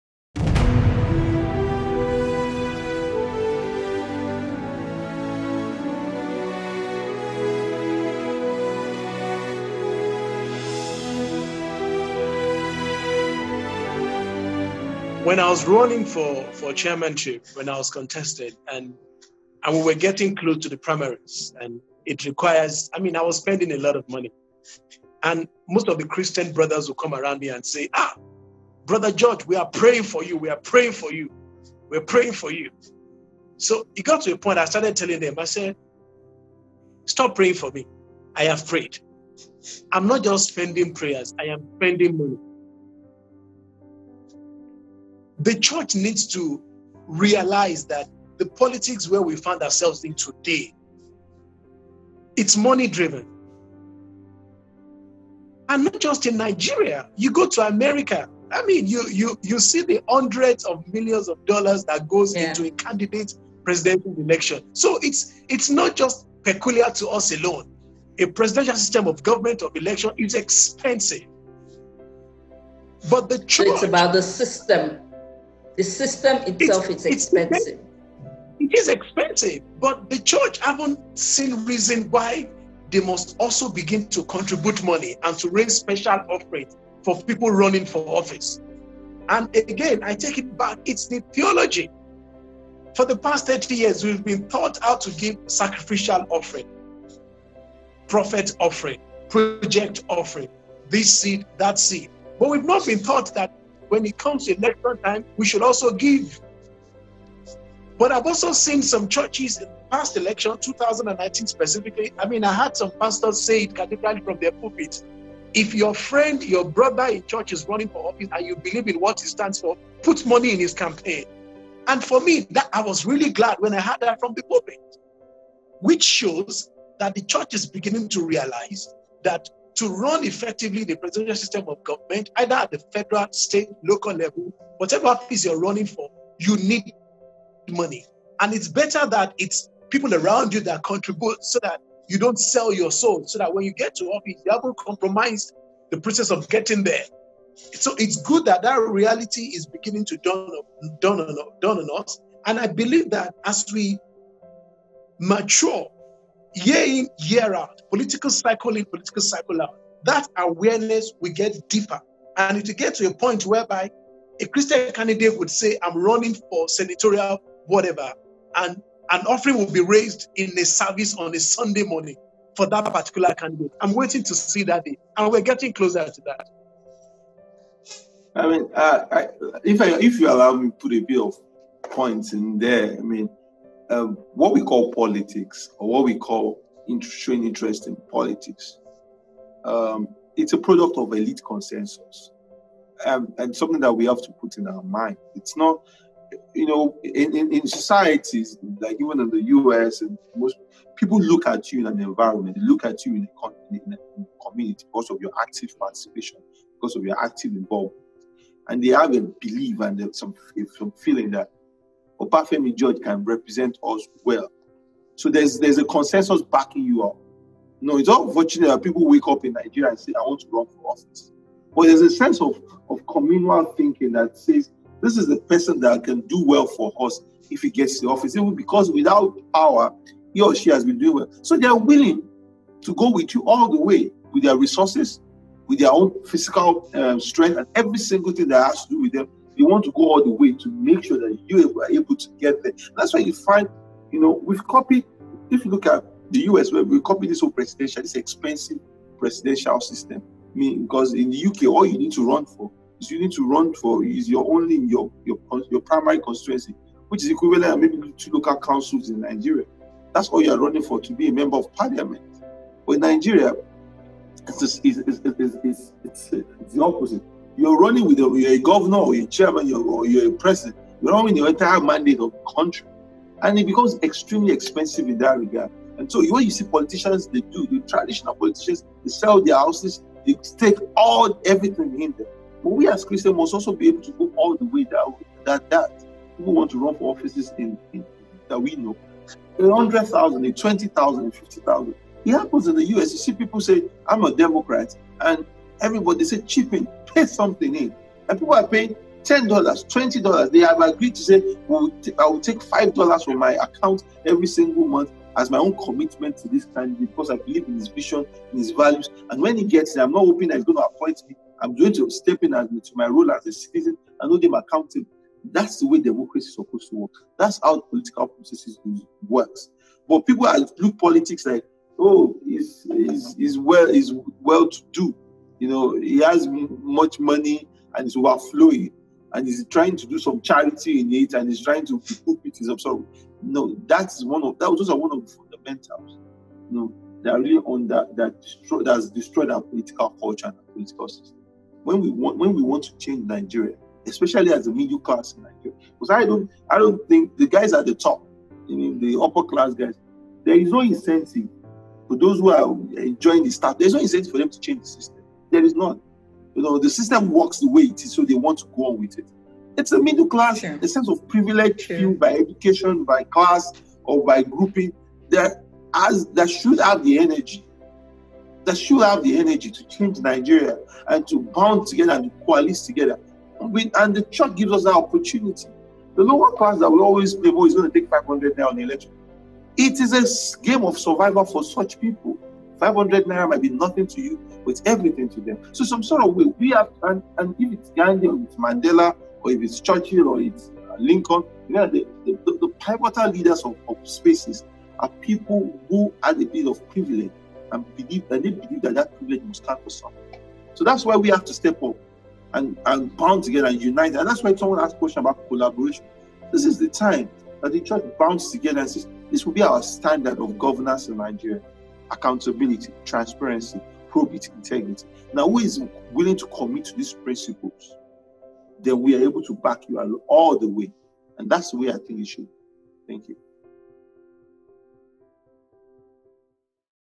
THE CHURCH AND POLITICAL FUNDING .
The Church and Political Funding by Oluseun Onigbinde (Co- Founder BudgIT) and Obafemi George ( Political Scientist ).